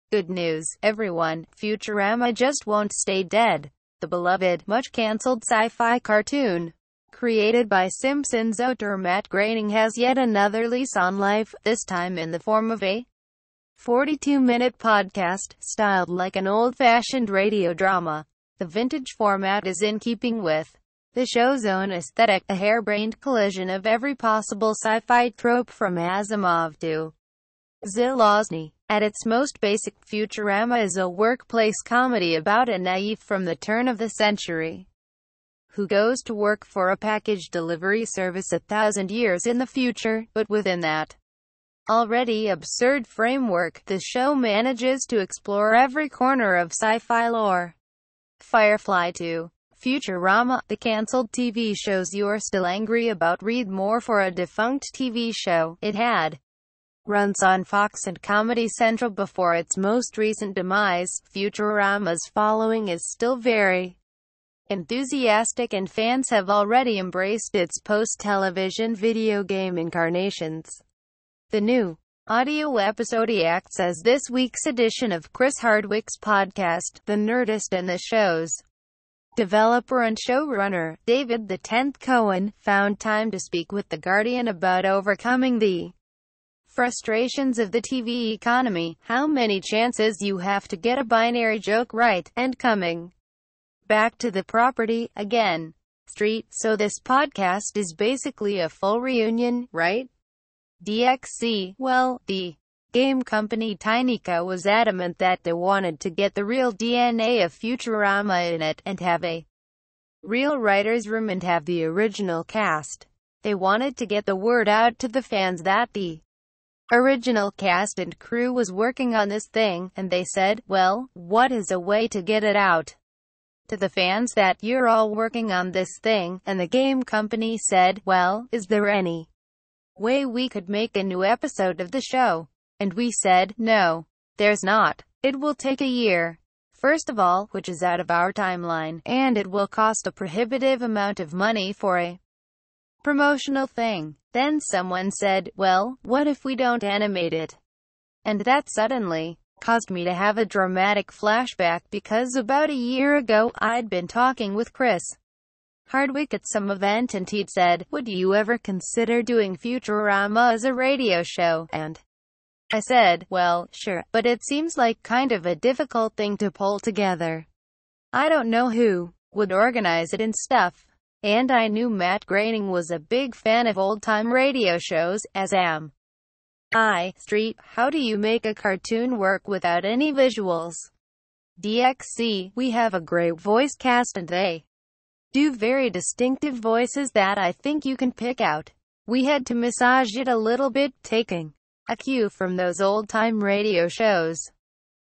The Future Is Radio Why Fan Favorite Futurama Moved To Audio Only
--  Good news  everyone  Futurama just won t stay dead.  The beloved  much canceled sci fi cartoon created by Simpsons auteur Matt Groening has yet another lease on life  this time in the form of a 42 minute podcast  styled like an old fashioned radio drama.  The vintage format is in keeping with the show s own aesthetic  a harebrained collision of every possible sci fi trope from Asimov to Zelazny.  At its most basic  Futurama is a workplace comedy about a naif from the turn of the century who goes to work for a package delivery service a thousand years in the future  but within that already absurd framework  the show manages to explore every corner of sci fi lore.      Firefly to Futurama  the cancelled TV shows you are still angry about           Read more       For a defunct TV show   it had runs on Fox and Comedy Central before its most recent demise   Futurama s following is still very enthusiastic and fans have already embraced its post television video game incarnations.  The new audio episodeacts as this week s edition of Chris Hardwick s podcast  The Nerdist and the show s developer and showrunner  David X Cohen  found time to speak with the Guardian about overcoming the frustrations of the TV economy  how many chances you have to get a binary joke right  and coming back to the property   again. ST  So this podcast is basically a full reunion  right  DXC  Well  the game company TinyCo was adamant that they wanted to get the real DNA of Futurama in it  and have a real writers  room and have the original cast.  They wanted to get the word out to the fans that the original cast and crew was working on this thing  and they said   Well  what is a way to get it out to the fans that you re all working on this thing   and the game company said   Well  is there any way we could make a new episode of the show   And we said   No.  There s not.  It will take a year  first of all  which is out of our timeline  and it will cost a prohibitive amount of money for a promotional thing.   Then someone said   Well  what if we don t animate it   And that suddenly caused me to have a dramatic flashback  because about a year ago I d been talking with Chris Hardwick at some event and he d said   Would you ever consider doing Futurama as a radio show   and I said   Well  sure  but it seems like kind of a difficult thing to pull together.  I don t know who would organize it and stuff.   And I knew Matt Groening was a big fan of old time radio shows  as am I.  ST  How do you make a cartoon work without any visuals  DXC  We have a great voice cast and they do very distinctive voices that I think you can pick out.  We had to massage it a little bit  taking a cue from those old time radio shows.  We decided to add a narrator to fill in any gaps that were things you would have seen on the air.             Facebook        Twitter        Pinterest    ST  Is this going to be a regular thing  It seems like the reaction has been pretty positive.  DXC  I think the question is what the timing would be if they wanted to do it again   maybe when they were going to unleash something new in the game--